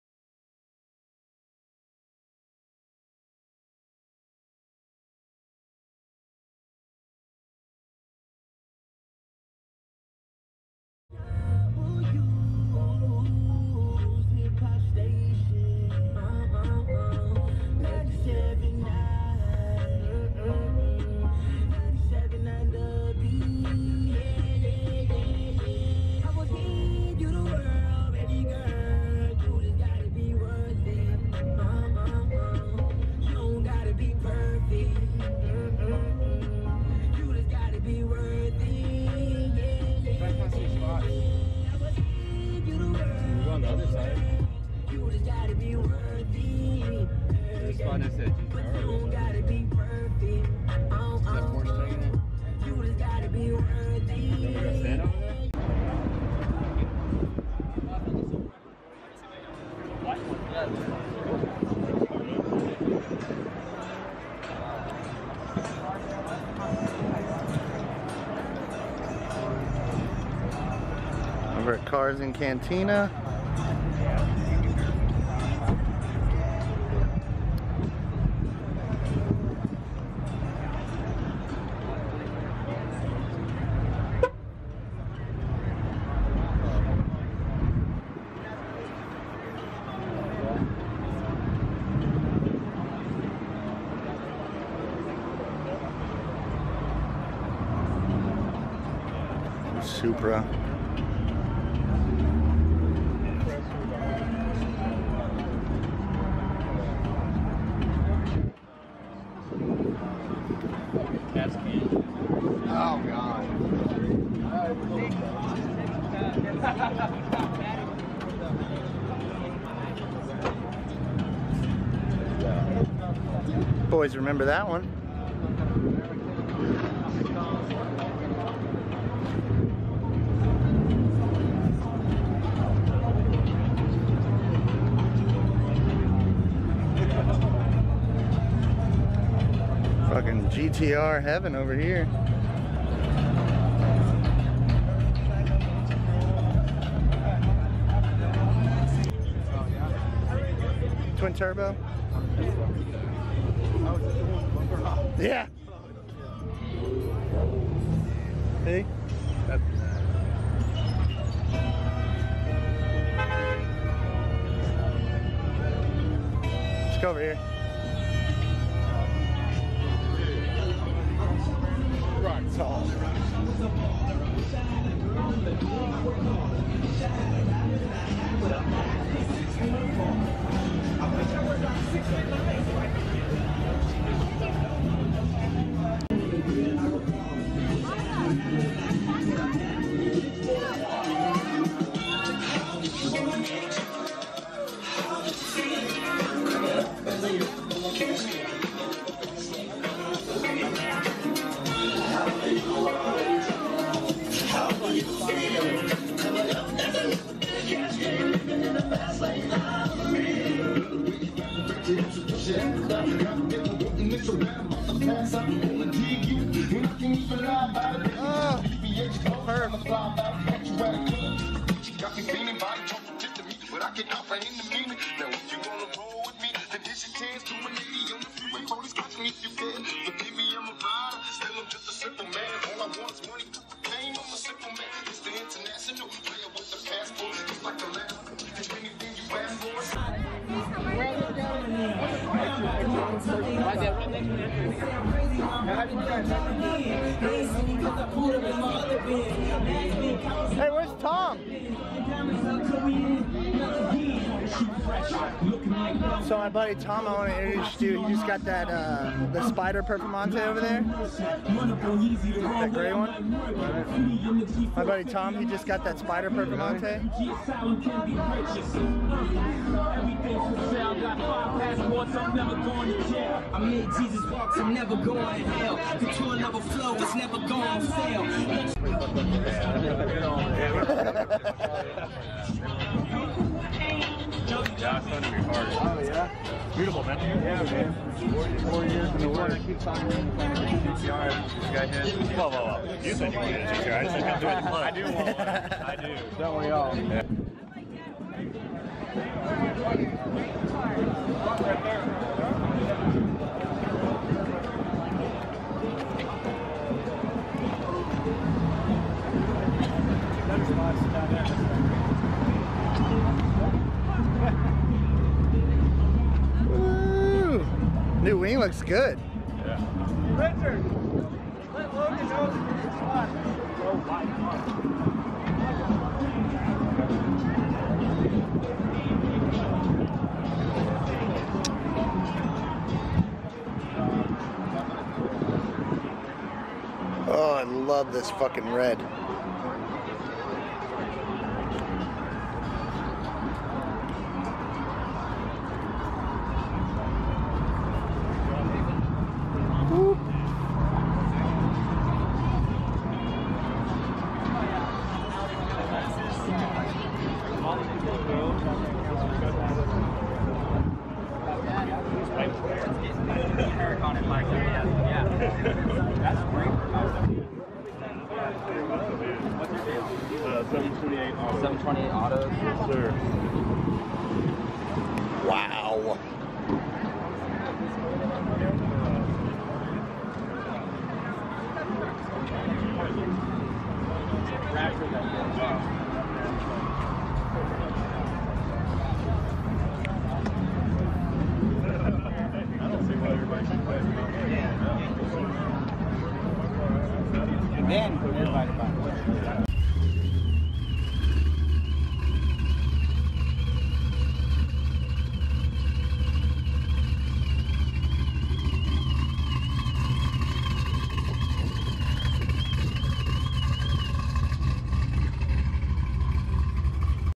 Cars and cantina saturday with the boys
park up front event at lava cantina … local meet up for car lovers